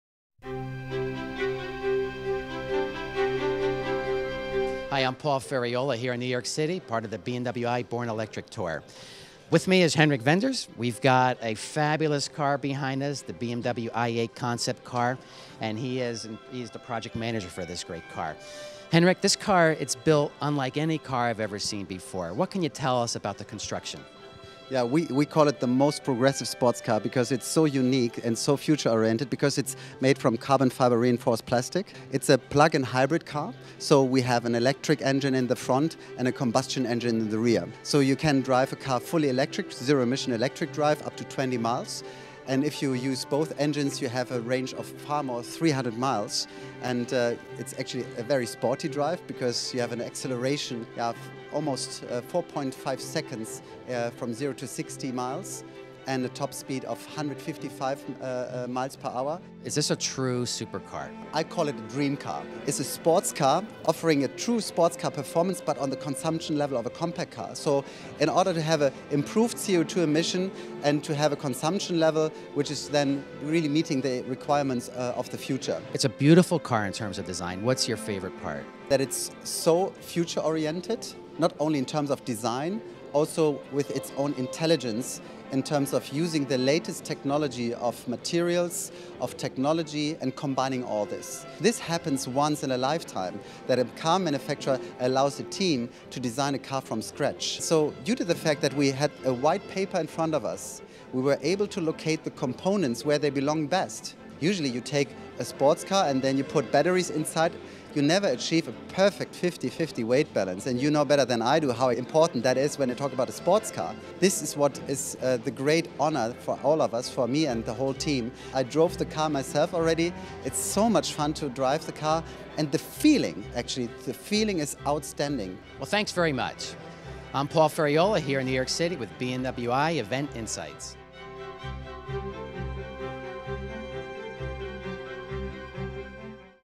Event Insights at the BMW i. BORN ELECTRIC TOUR. NYC. - Henrik Wenders, BMW i8 Project Manager
This version of the BMW i8 Concept made its first appearance in the US as part of the BMW i Born Electric Tour. Henrik Wenders, head of BMW i8 Concept product management, discusses the design, build and eco-friendly features of the i8 Concept with Paul Ferriaiolo. Follow our "Event Insights" videos for more interviews discussing the making of the cars, release dates and the future of purpose-built vehicles.

The Future Is Closer Than You Think.